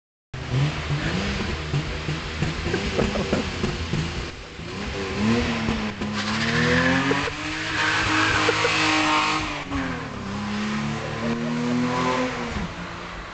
Treff der Szene - Burnout BMW with 400'000km!!
Jeden Mittwoch Treff der TuningSzene im COLOSSEO in Münsingen!